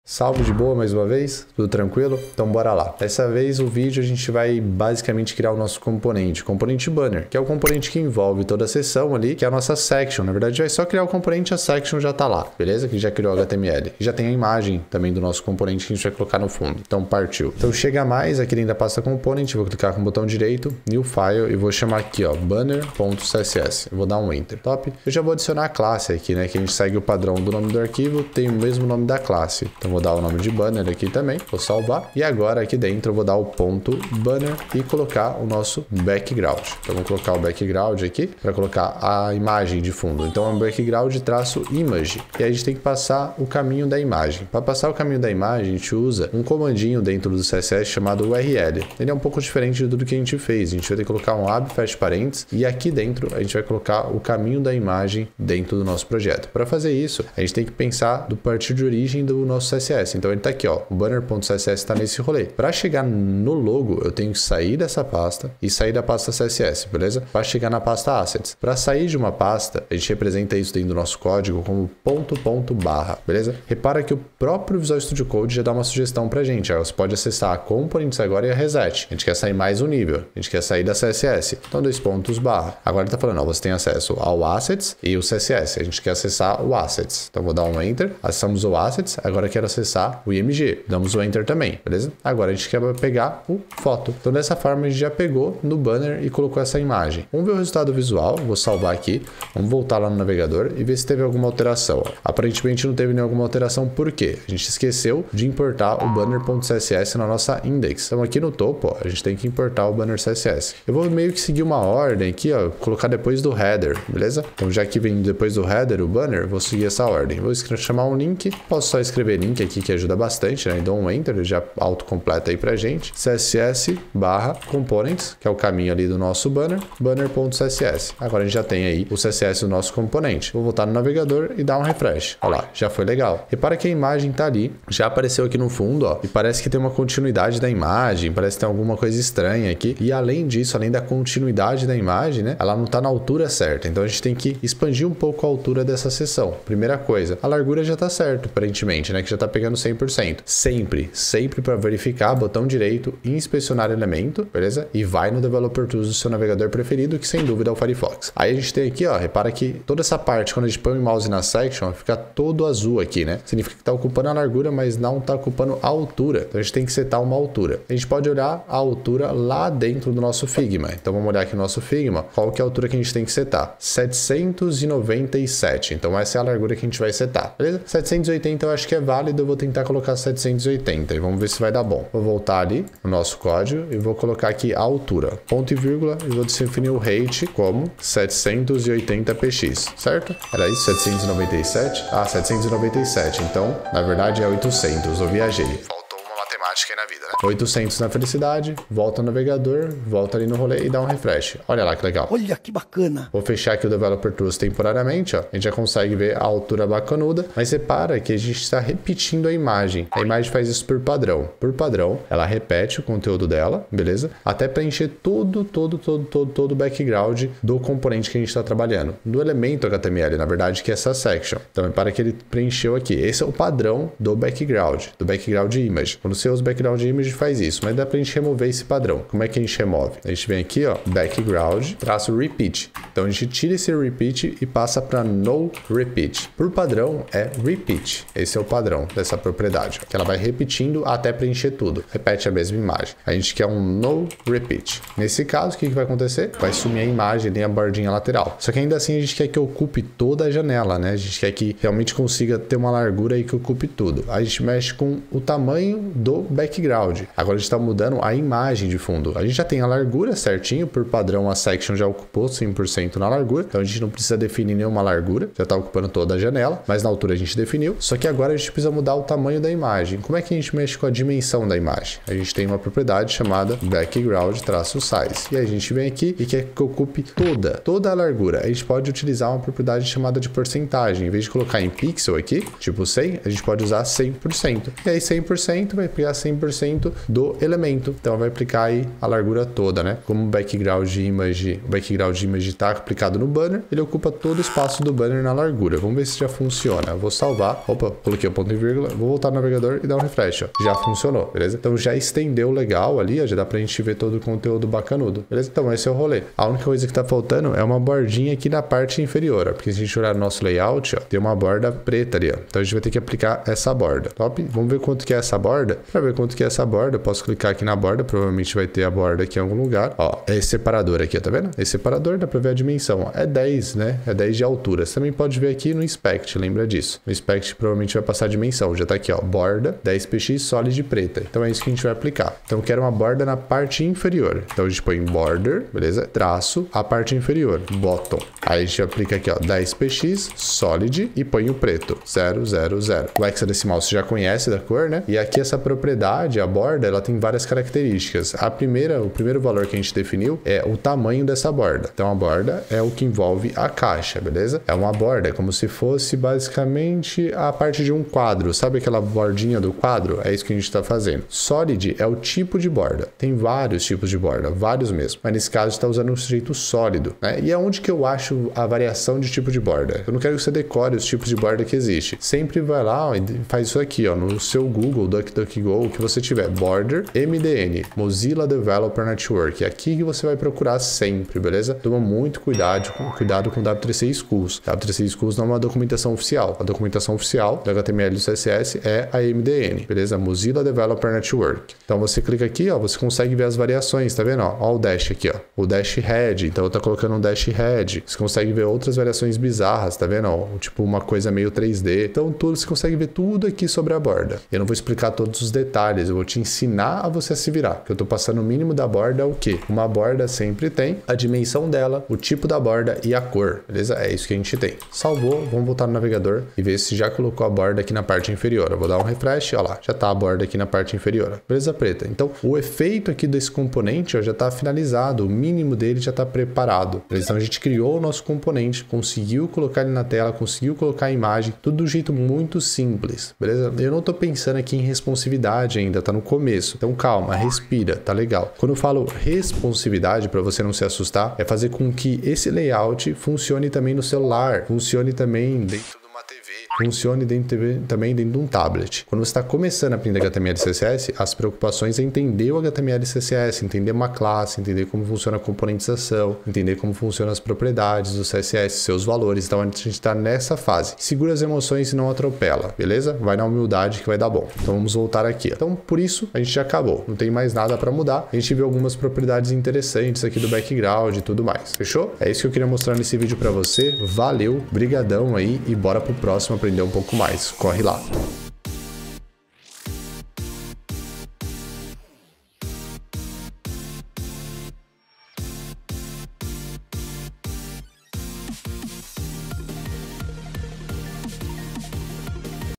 Estilizando o banner | Mód03 - Aula 22
Feliz e suave, booooraaa criar nosso component banner, que é o component que envolve toda a seção principal da página. Já criamos o HTML, hoje vamos estilizar os elementos e inserir a imagem de fundo. Ainda explico um pouco sobre o que é responsividade.